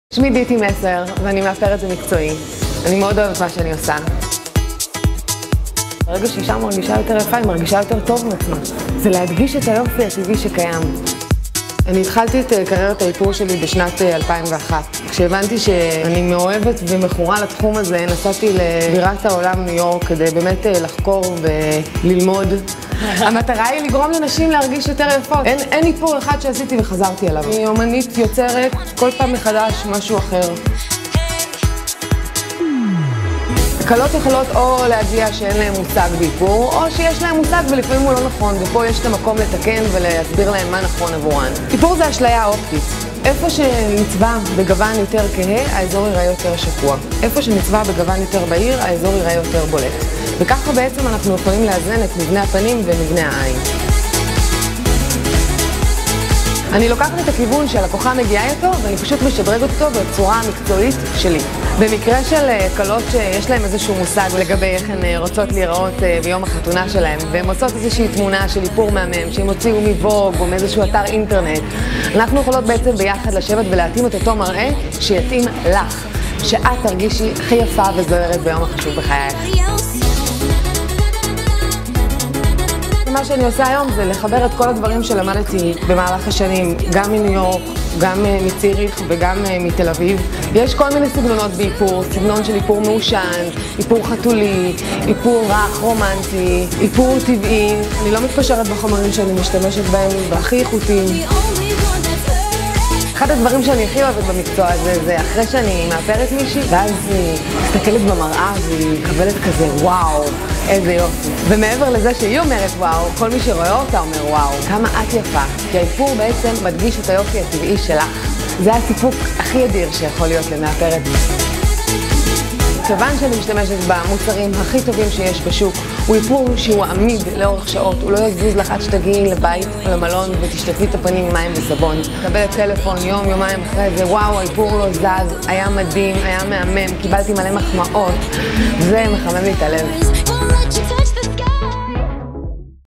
הצצה להפקת אופנה ואיפור של דיתי מסר
ביוטי מחוספס
הפקת קולקציית איפור סתיו 2011 של המאפרת דיתי מסר.

לוק 1: שפתיים מודגשות באדום מט. השראה מנשים חזקות ועצמאית בשנות ה-30.
לוק 2: מראה אייליינר רך ורומנטי. מושפע מהנשיות המתפרצת של שנות ה-50.
לוק 3: מעושן סקסי. מראה מעושן קלאסי, מגדיל את העין ויוצר מראה חתולי.
לוק 4: אצילי. רכות מלטפת בנגיעות בגוון זהוב וברונזה שמשרה מראה רך וחד כאחד.
לוק 5: עיניים מעושנות בשחור מודגש. מראה עמוק ומדוייק.

הפקה ואיפור: דיתי מסר
דוגמנית: יבה דון
עיצוב שיער: ליאור אסולין
סטיילנג: קורין גולד מגדים
ביגוד: מרדכי אברהם
צילום סטילס: אלכס ליפקין
עוזרת מאפרת: סופי
צלם וידאו: רומן פלוטקין